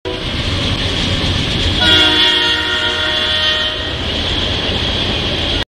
Kieran's Trains July 20 2017
Daily Video Blog for Chesterton, IN rail traffic